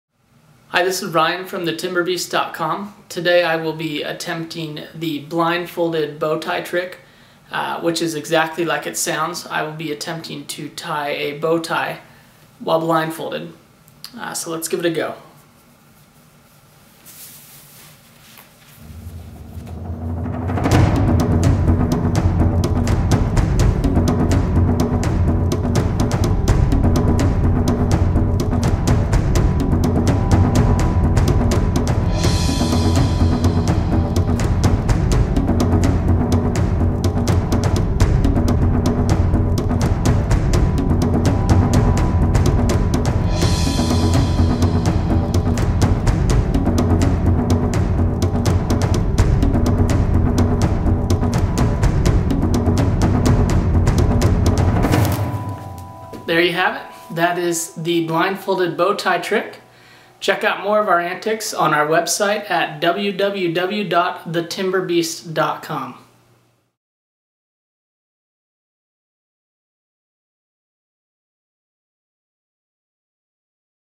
Blindfolded Bow Tie Trick - TimberBeast
TimberBeast Co-Founder Ryan attempts the blindfolded bow tie trick.